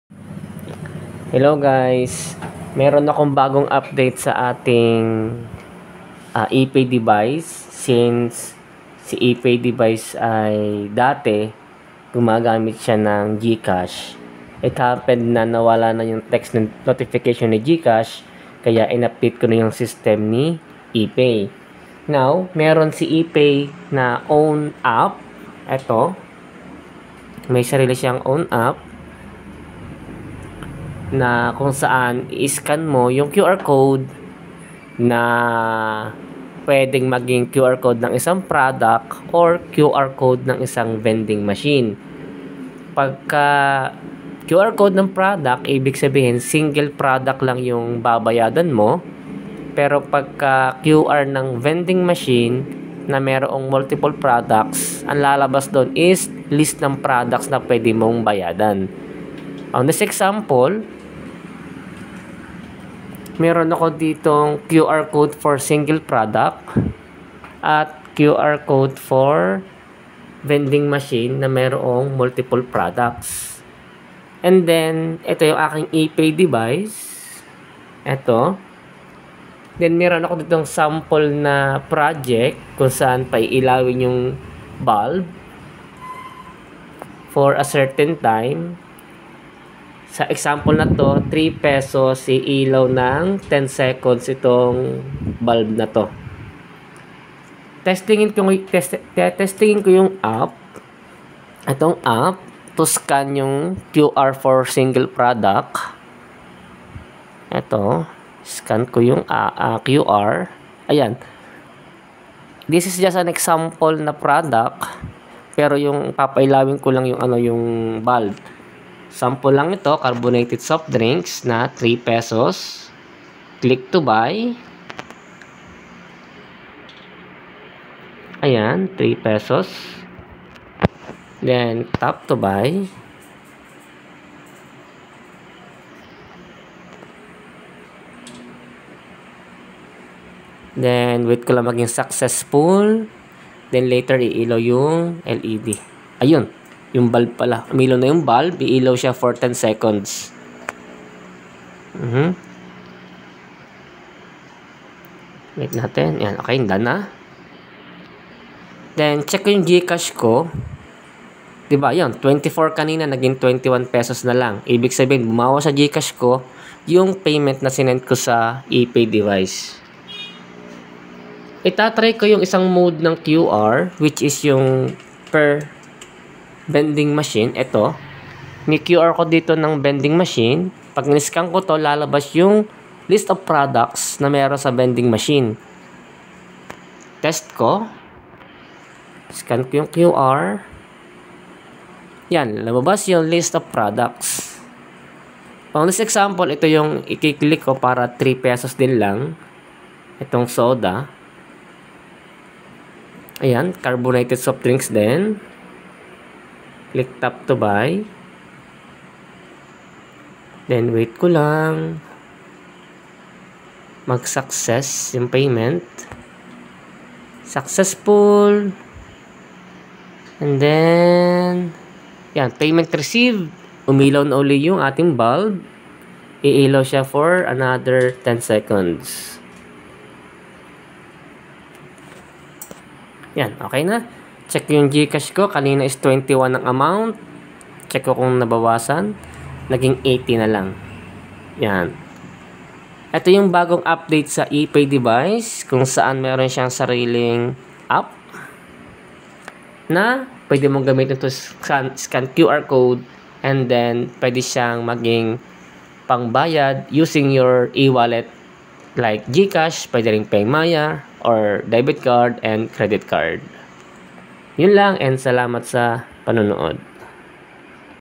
Vending Machine Using GCash, Paymaya, Debit, Credit Card, Paypal and ShopeePay.
This is an experimental new feature of E-Pay device. The device has it's own app that use your google play's  payment methods therefore you payment can be charge to your GCash, Paymaya, Debit/Credit card, Paypal and ShopeePay.

As of now, you can use the e-pay device to your projects that use serial communication. It can be modified to use MDB protocol if needed.